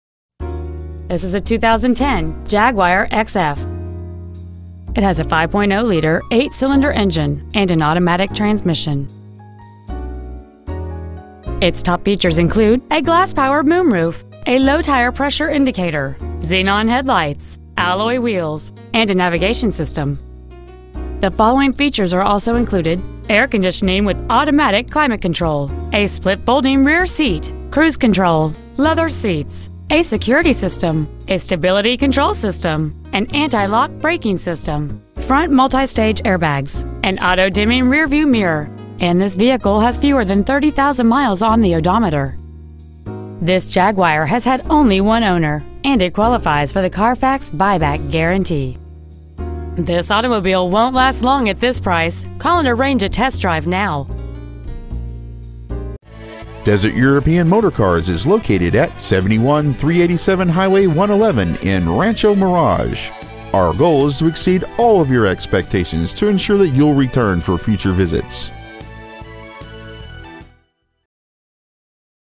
2010 Jaguar XF Rancho Mirage CA 92270
We are proud to present this 2010 Jaguar XF .

 Year : 2010
 Make : Jaguar
 Model : XF
 Engine : 5.0L V8 engine
 Trans . : Automatic
 Exterior : Black
 Miles : 29,781
 Interior : Other
 Stock : X2417A